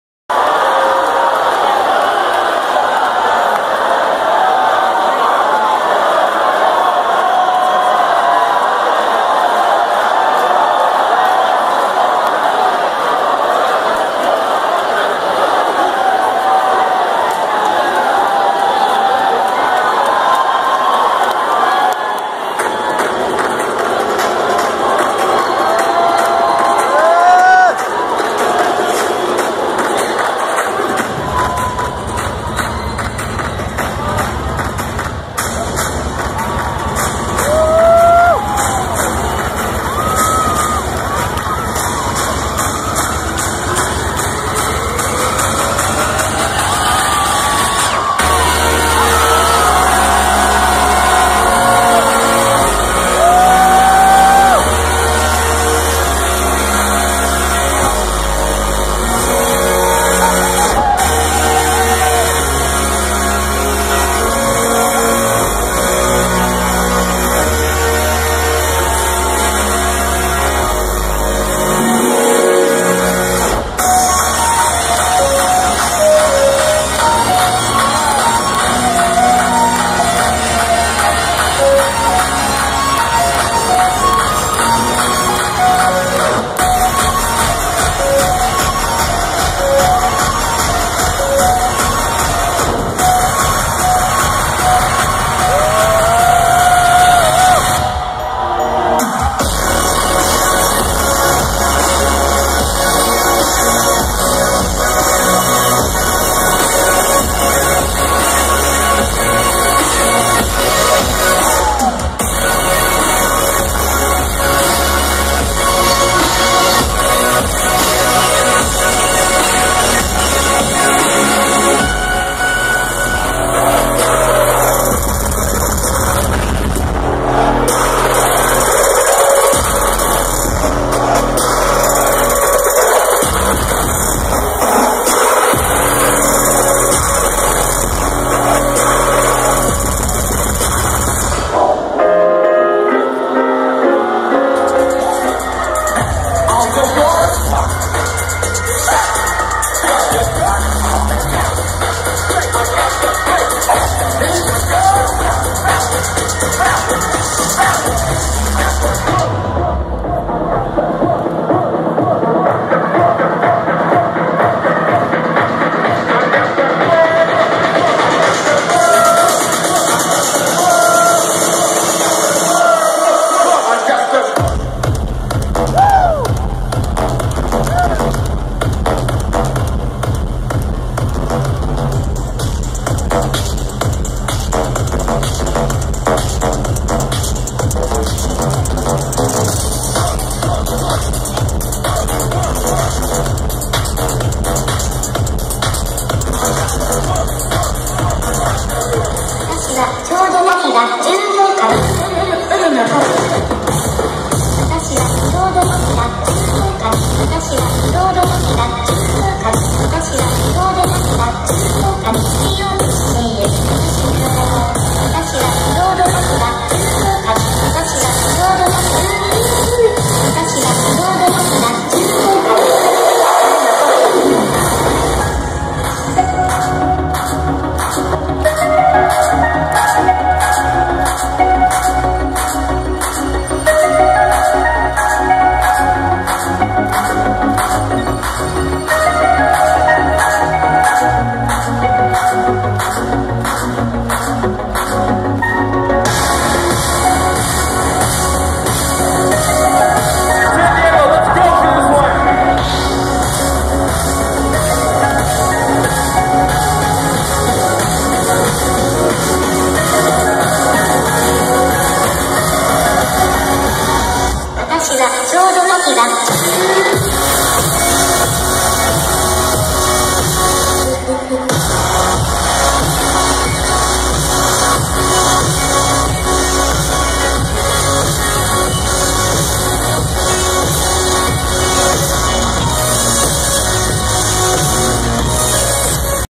Porter Robinson @ Heaven & Hell 2015 San Diego
Porter Robinson dropping a 2 hour set for us on Halloween night in San Diego. He went hard af